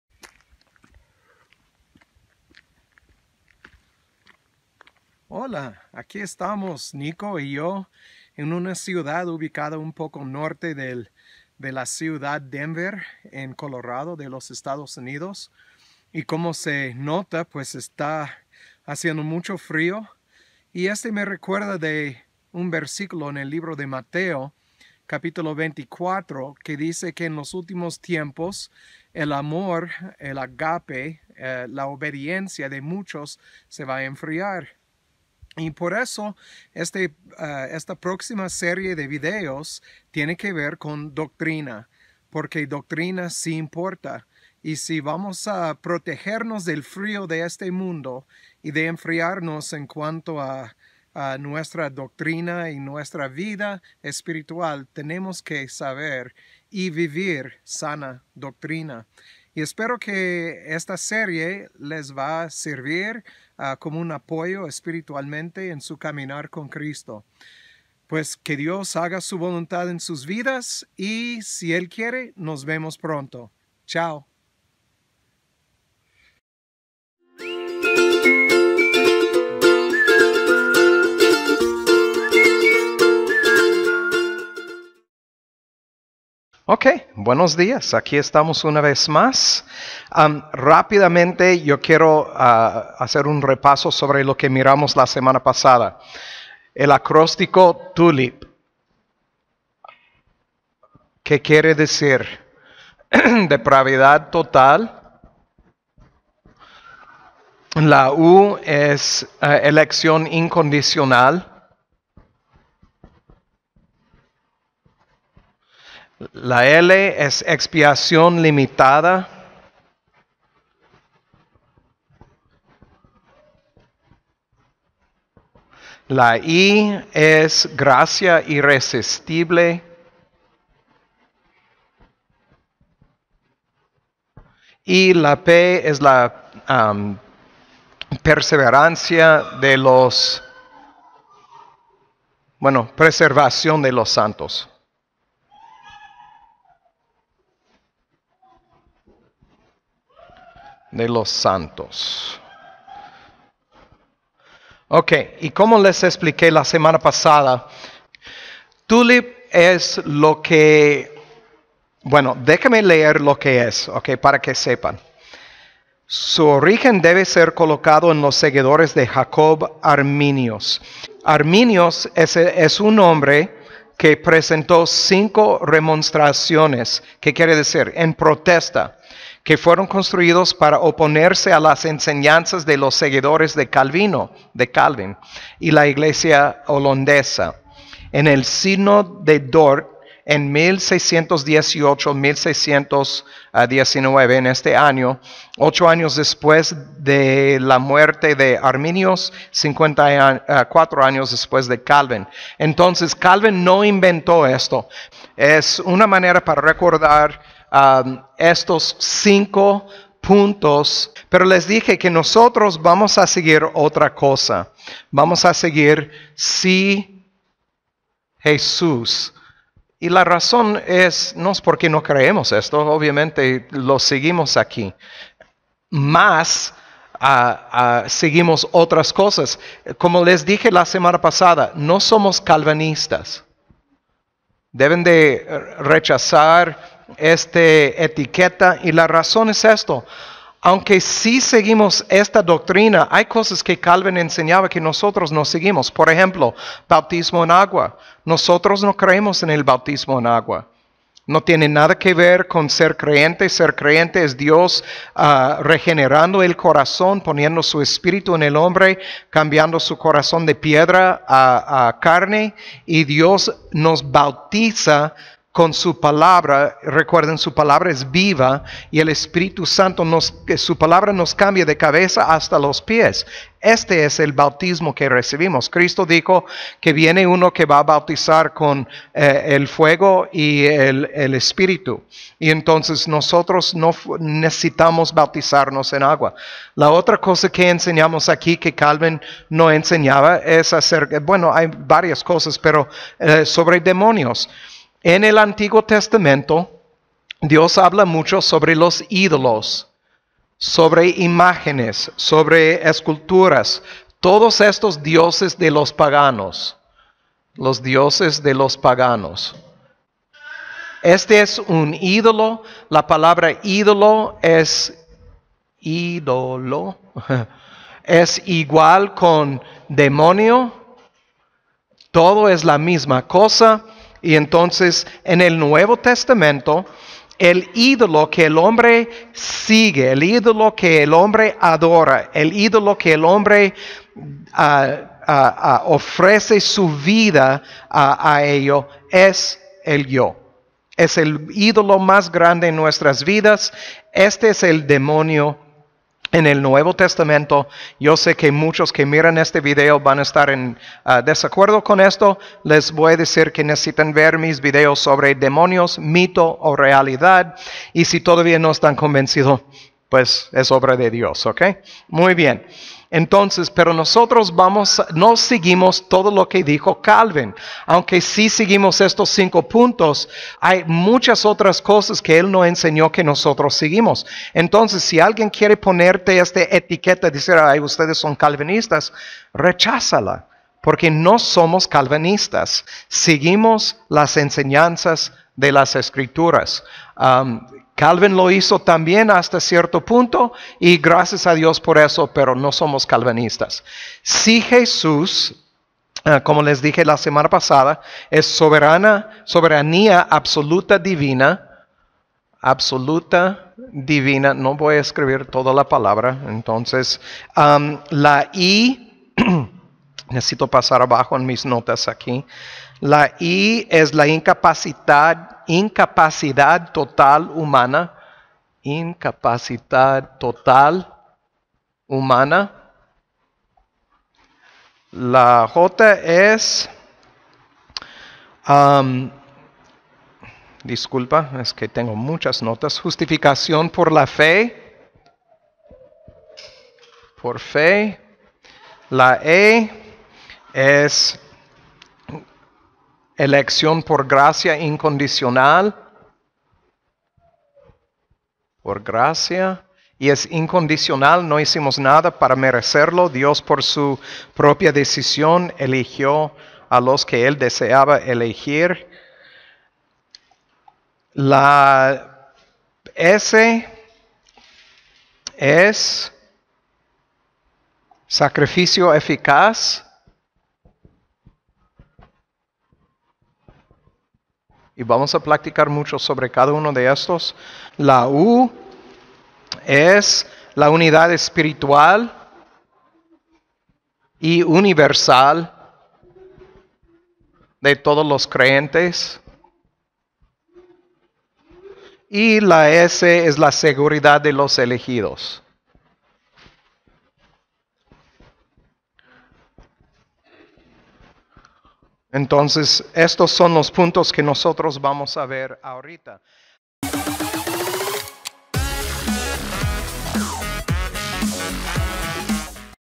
SI001- El Acrostíco SI JESÚS (la serie fue actualizada. veáse abajo para el linc)
Este video es la primera de una serie sobre el acróstico SI JESÚS. Partes del estudio vienen de un estudio hecho por Roger Smalling. Un hermano que ha publicado muchos libros sobre doctrina en español. Se puede bajar el estudio sobre sana doctrina y

Cada video en la serie ha sido editado para quedarse dentro de un tiempo breve. Entonces se debe de verlos en ordén o no va a tener sentido la serie (por eso se llama serie :) ).

Sé que hay los que usan el ejemplo de títeres cuando hablan del control que Yehová Dios tiene sobre la gente y no es que estoy en contra de este tipo de ejemplo nada mas prefiero no usar algo que los autores de la biblia no dicen. En Las Escrituras es bien claro que somos el barro y Dios el Alfarero. Por eso lo dije como lo dije en el video, que casí son iguales - pero no son iguales. Creo que es una cuestión de semántica.

Mi deseo para ustedes que están viviendo la verdad es que Dios les mantiene en el camino estrecho seguiendo la verdad.